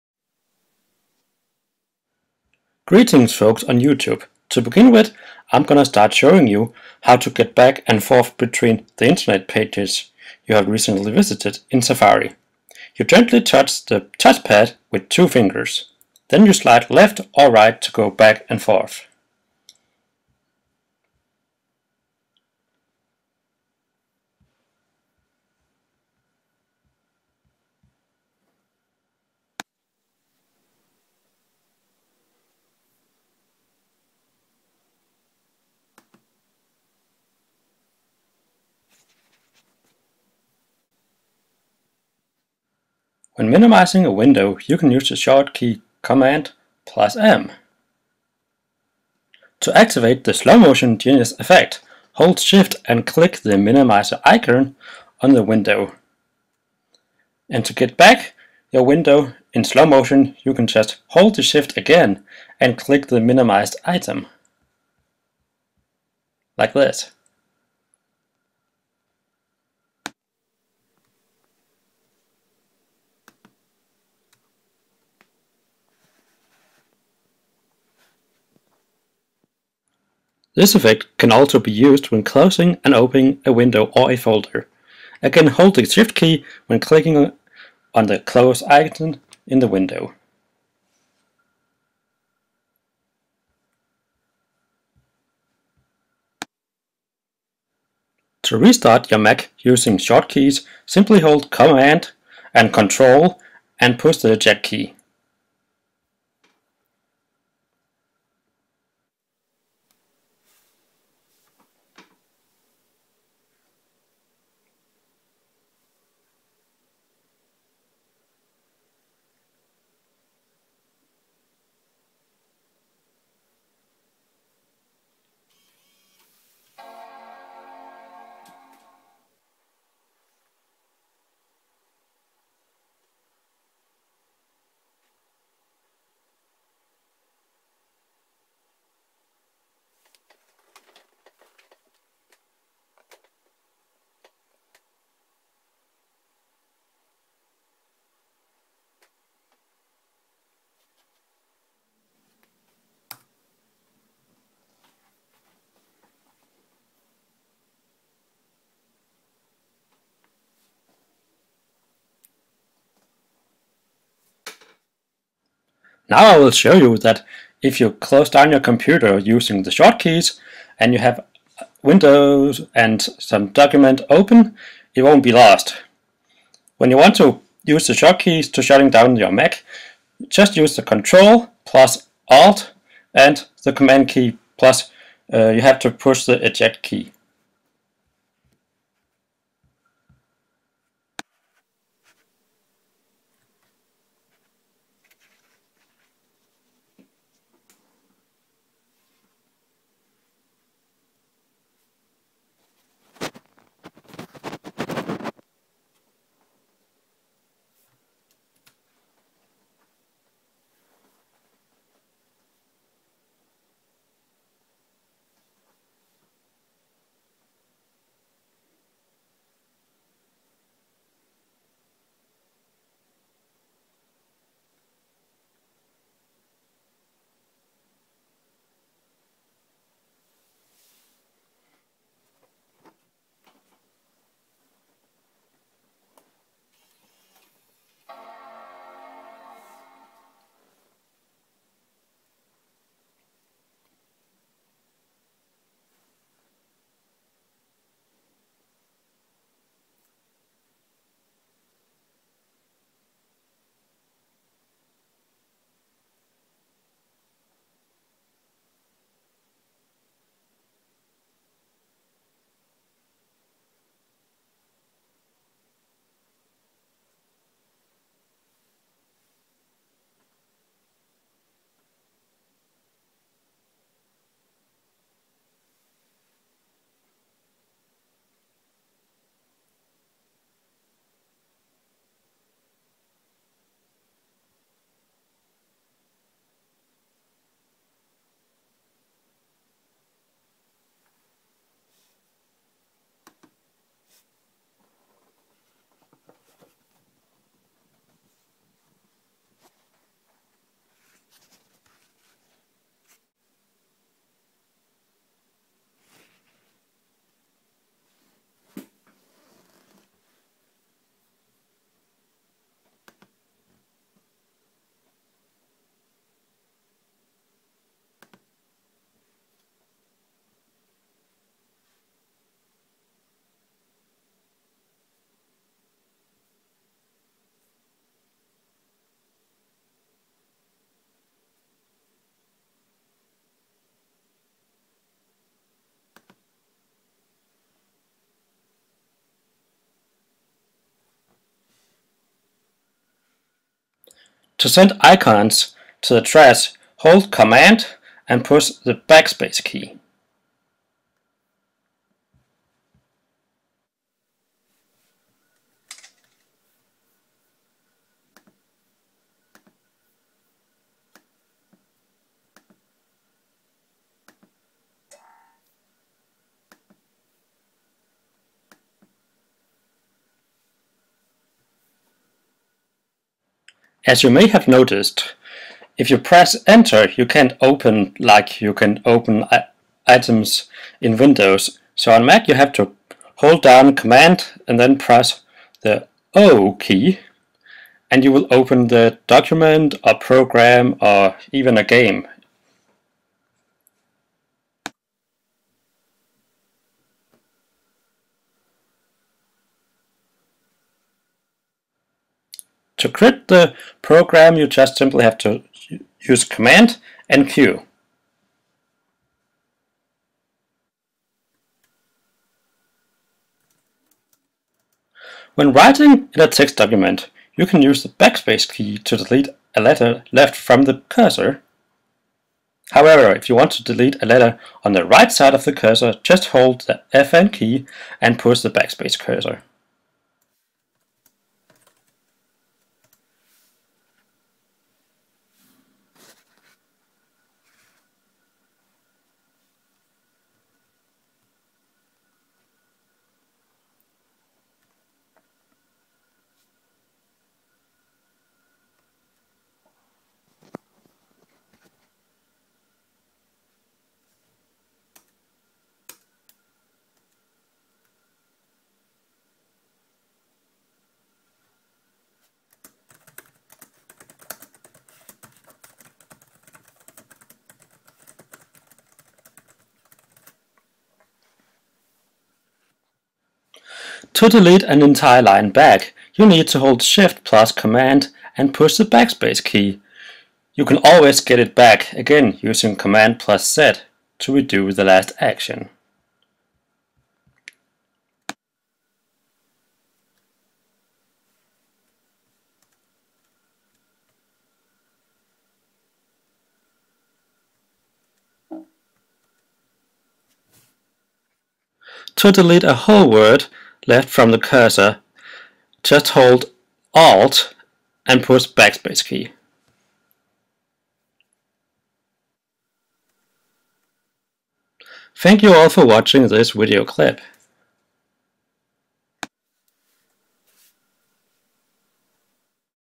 Mac tricks and shortcut keys
Showing some useful shortcut keys and tricks on mac